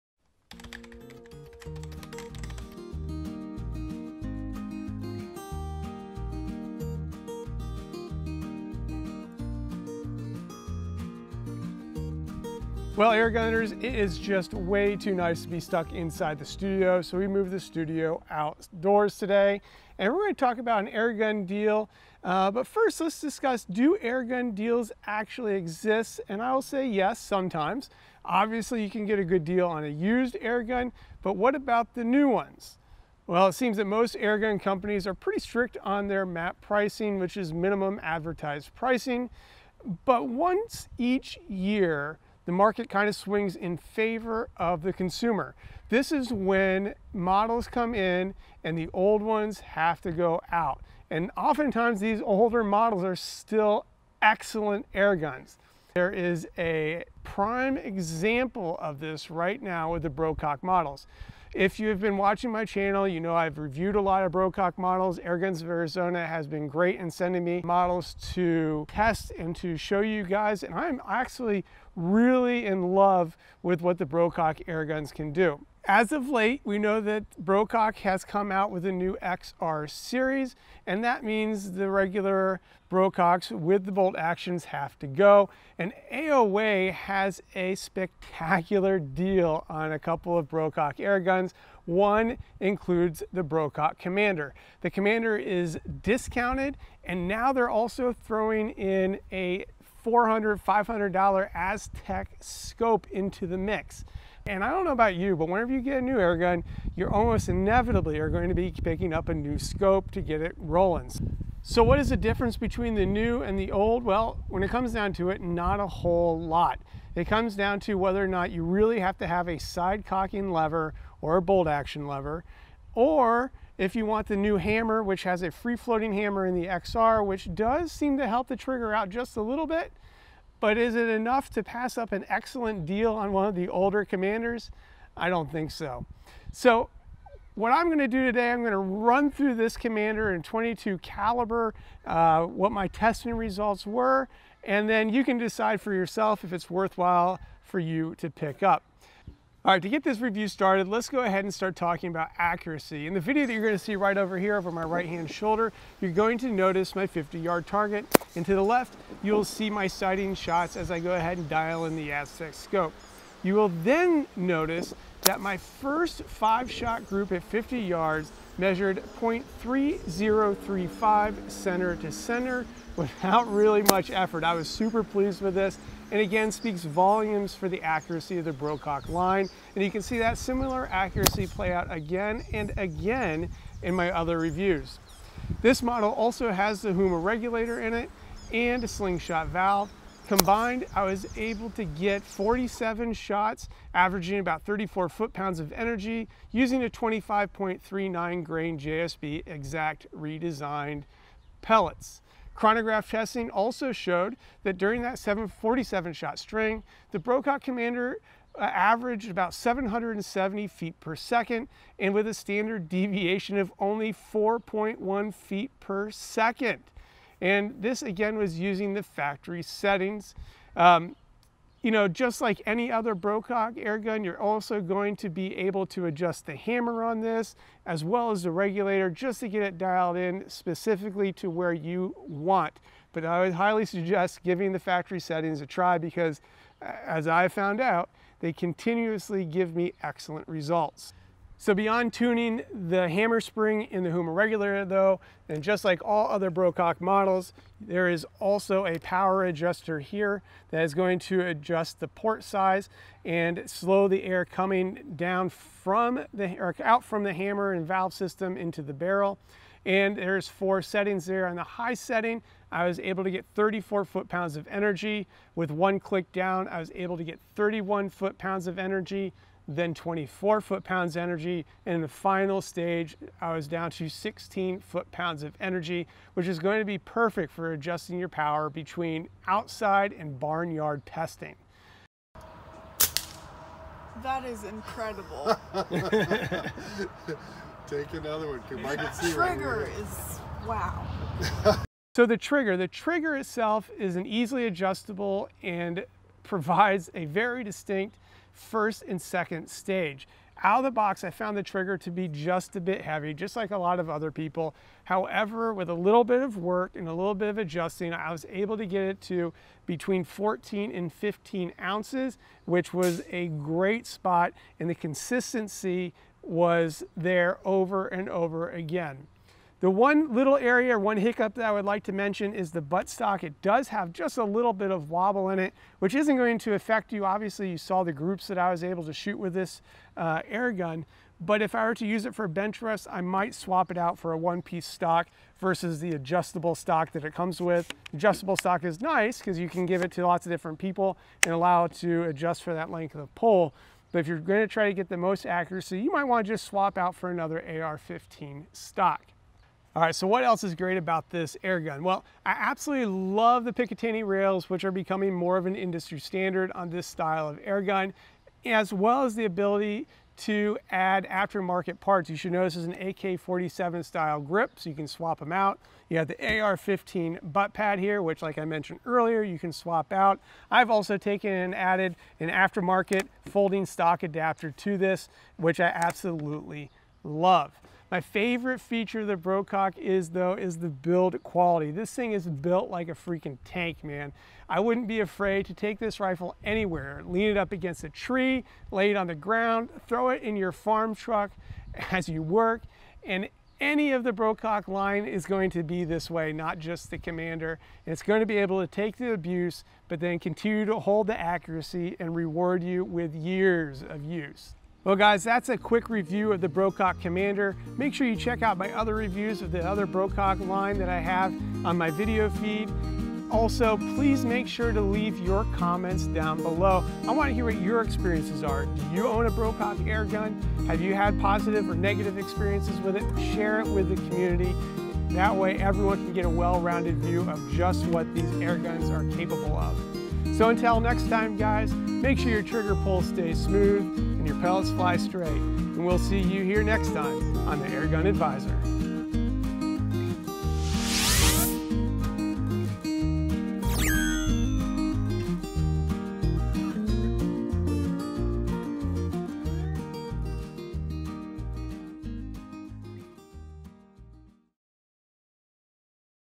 Airgun Advisor: DEAL ALERT Brocock Commander
Anyone else notice my typo? Should say Brocock XR not XP... 🤦🏼‍♂️ I’m going to fire my editor. 😅

Airgun deals, do they exist? Well yes, sometimes….

Obviously you can get a good deal on a used airgun, but what about the new ones? It seems that all airgun companies are fairly strict on their MAP (Minimum Advertised Price) Pricing. But once each year the market swings in favor of the consumer. This is when the new models come in and the older (but still excellent) models need moved out.

It just so happens, that there is a prime example of this right now with Brocock Models. If you have been watching my channel you should know that the new Brocock XR models are out and shipping.

What that means is that you can get a killer deal on an airgun that is just as accurate and exciting for a great deal. In front of me is the Brocock Commander Hi Lite which AoA and other retailers have discounted significantly. AoA has gone one step further and is now including a FREE Aztec Scope with the purchase.

What is different between the new and the old? Not much really other than  It really comes down to if you have to have a side cocking lever or don’t mind a more traditional bolt action. The only other significant change is the hammer in the XR which is now free floating. Which actually seems to make the trigger just a hair better than before.

I'm going to quickly run down the specs from my testing on the .22 caliber Brocock Commander Hi-Lit that I have in front of me and would like to thank Airguns of Arizona for sending to me for review. Also if you happen to be interested in one of the Brocock models make sure to checkout my reviews on the Brocock Sniper and Compact XR I tested previously.

Let us start by talking about accuracy. In the video you see my sighters to the left as I dialed in the Aztec scope. You will then notice that the first five shot group at 50 yards measured .3035 CTC without much effort! It really speaks volumes for this and other Brocock airguns.

Again, this model has the world renowned HUMA regulator and slingshot valve. Combined I was able to get 47 shots averaging 34 FT-Lbs of energy using the 25.39 grain JSB Exact Redesigned Jumbo Monsters. Chronograph testing also showed that during the 47 shot string, the Brocock Commander averaged 770 fps with a standard deviation of ONLY 4.1 fps.   The testing was completed using the factory setup and as you know from my other videos, you can easily adjust both the regulator setting and hammer spring tension to tune it exactly how you want.

Like all current Brocock airguns you can adjust the power by the quick power adjuster knob. This will then adjust the port size, slowing the release of air from the valve to the barrel.  My testing showed power levels starting with 34 ft-lbs and then stepping down with each turn of the knob to 31 ft-lbs, 24 ft-lbs and 16 ft-lbs. Perfect for adjusting your power between outside to barnyard pesting.

The trigger is easily adjustable and provides a very distinct first and second stage. Out of the box I found the trigger to be a bit heavy. So I did a little work and was able to adjust the trigger so it averaged between 14-15 oz. and like other Borcock models I found it to be consistent time and time again.

The butt stock is solid and you have to love the adjustability. The only criticism I have is that it does have a little play due to the adjustability, but nothing serious unless you would be looking to shoot Benchrest. Then I might eventually consider swapping it with a non adjustable stock.

So what else is great about this airgun? I absolutely love the picatinny rails which seem to be becoming an industry standard and the ability to add aftermarket parts like various AK-47 grips and your favorite AR-15 butt stocks. So far, I’ve only added an aftermarket folding adapter, which I would recommend. My favorite feature is the Brocock build quality. It is built like a tank and I wouldn’t be afraid to take this rifle anywhere. Lean it against a tree, lay it on the ground or throw it in the farm truck as you work and any of the Brocock line will take the abuse and continue to reward you with accuracy.

If you guys have more questions or want to share your experience with a Brocock airgun, please make sure to leave your comments below and as always; may your trigger pull stay smooth and pellets fly straight and we will see here next time for the Airgun Advisor.